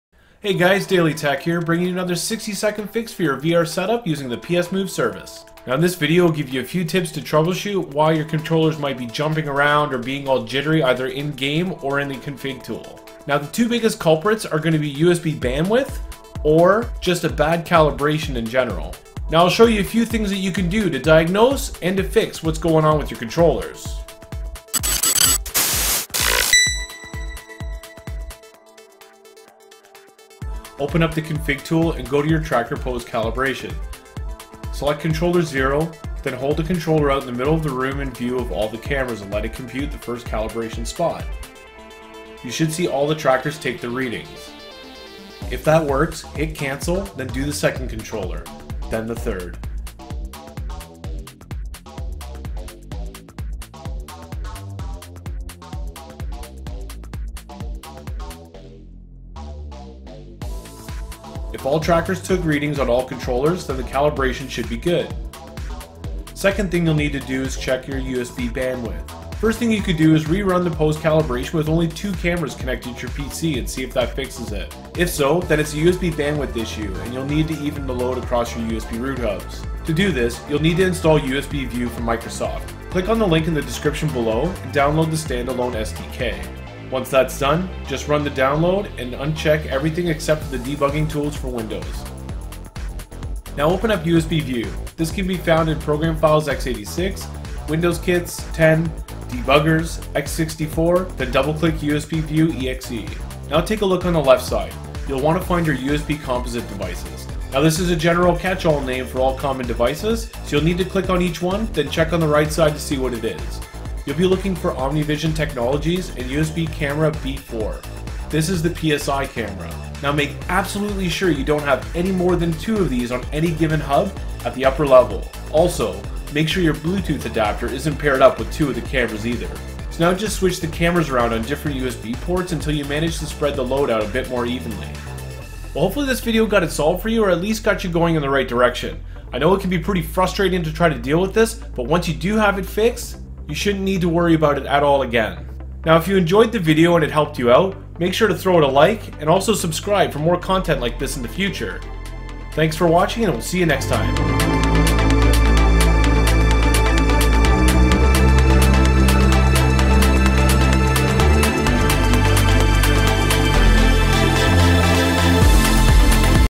How to Fix PS Move Controller Jitter and Crashes with PS Move Service with Steam VR
In this video, I will show you how to troubleshoot common problems that cause jittering controllers and other odd controller behavior.  This happens with 3-4 camera setups when USB gets overloaded or you have bad calibration.  This isn't ALL the fixes for it, but these are the ones that are the most common and easiest to fix.

I will have plenty more content out in the future!